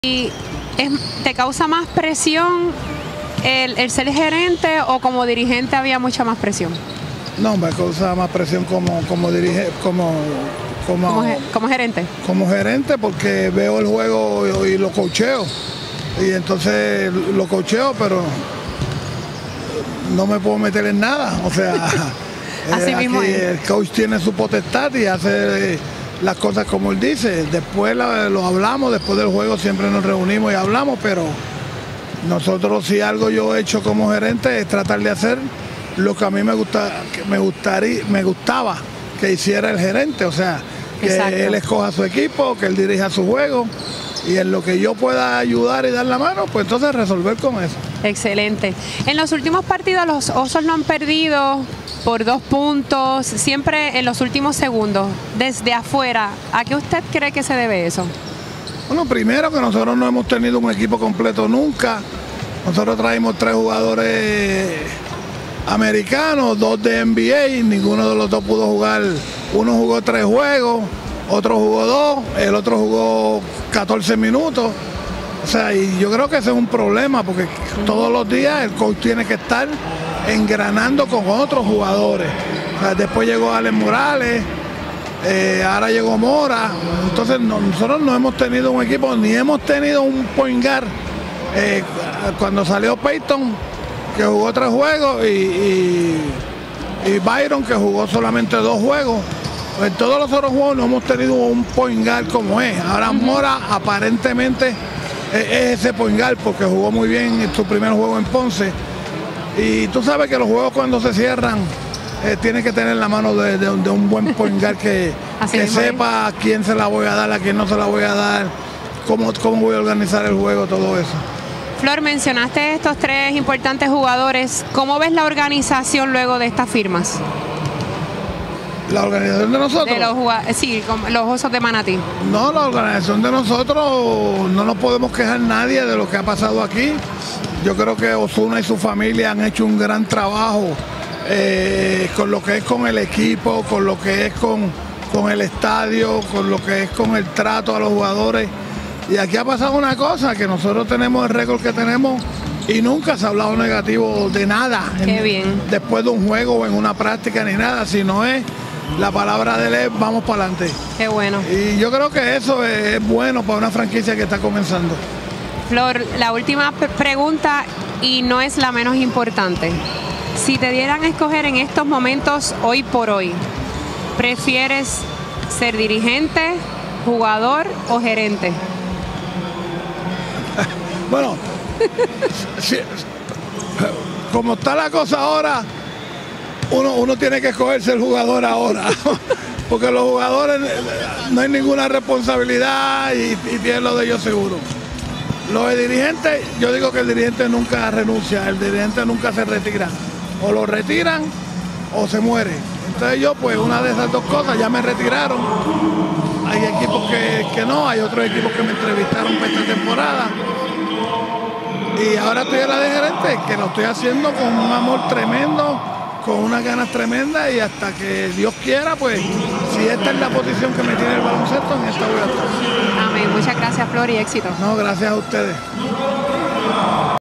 Flor Meléndez disfruta de su posición como gerente con los
Flor Meléndez…. Una leyenda de nuestro país…

Escuchen esta entrevista…¿prefieres el rol de jugador, dirigente o ferente?

Adivinen qué contestó. Video de la transmisión @OsosdeManati-ot2he